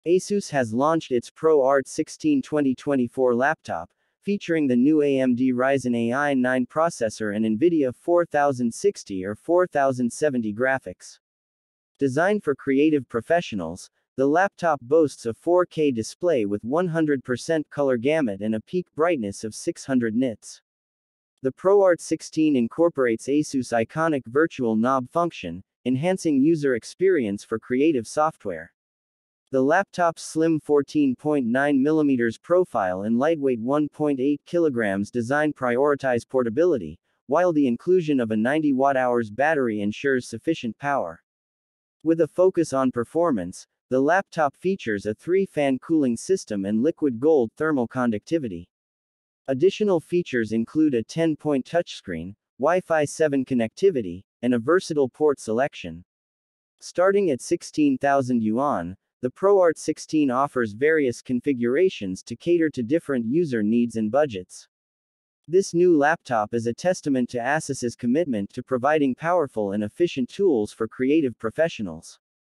ASUS ProArt 16 2024 Laptop with AMD Ryzen AI Processor, RTX 4060/4070 Graphics, at 15,999 Yuan
Asus has launched its Pro Art 16 2024 laptop, featuring the new AMD Ryzen AI 9 processor and Nvidia 4060 or 4070 graphics. Designed for creative professionals, the laptop boasts a 4K display with 100% color gamut and a peak brightness of 600 nits.